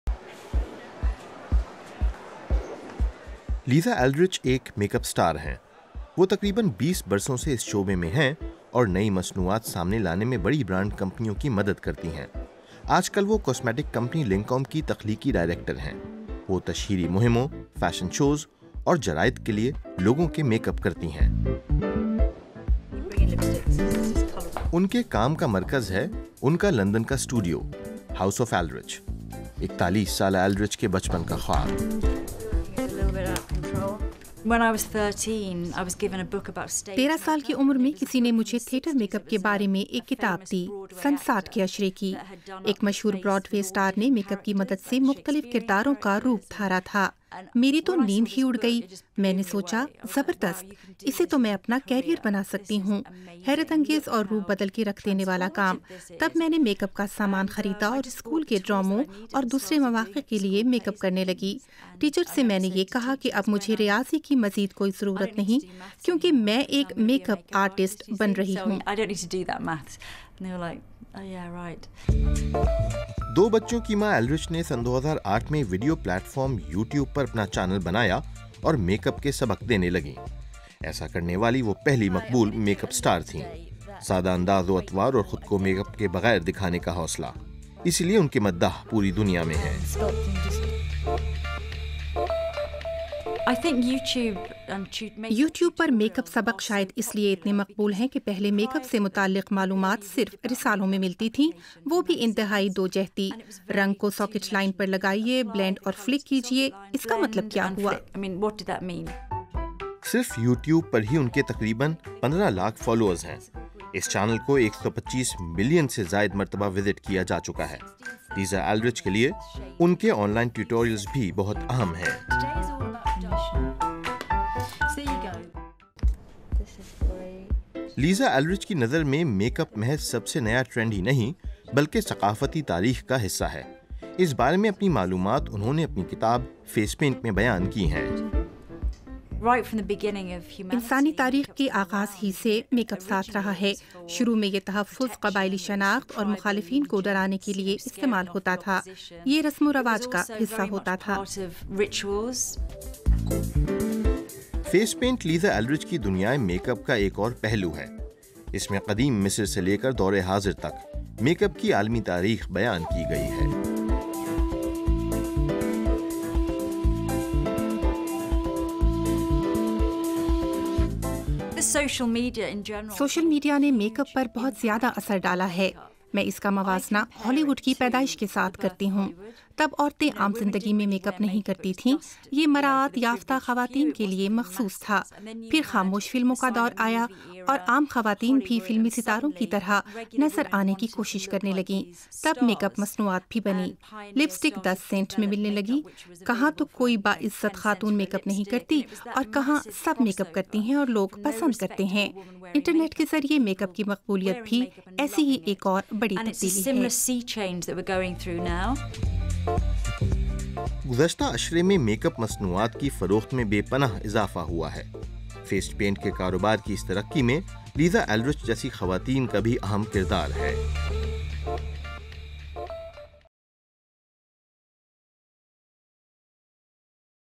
دنیا کی مشہور ترین میک اپ آرٹسٹ | Superstar Makeup Artist
برطانیہ کی لیزا ایلڈرچ دنیا کی مشہور ترین میک اَپ آرٹسٹ ہیں۔ سوشل میڈیا پر اُن کے فالوورز کی تعداد 3.4 ملین سے بھی زیادہ ہے۔ وہ کیٹ وِنسلیٹ یا کِیرا نائٹلی جیسی فنکاراؤں کا میک اَپ کرتی ہیں اور  ماریو ٹیسٹِینو یا پَیٹرِک ڈی مارشیلیے جیسے فوٹوگرافروں کے ساتھ مل کر کام کرتی ہیں۔ یُو ٹیوب پر اُن کے میک اَپ کے اسباق کو لاکھوں لوگ دیکھتے ہیں اور  وہ فرانسیسی کاسمیٹک کمپنی لَنکوم کے لیے نت نئی مصنوعات تیار کرتی ہیں۔ میک اَپ ہی لیزا ایلڈرچ کا اوڑھنا بچھونا ہے۔ کئی مہینوں کے صبر آزما انتظار کے بعد ہم نے بالآخر اس خاتون کے لندن اسٹوڈیو میں اُن سے ملاقات کی ہے۔
Lisa Eldridge is a professional make up artist working with many of the worlds top A-list celebrities, models, magazines and brands.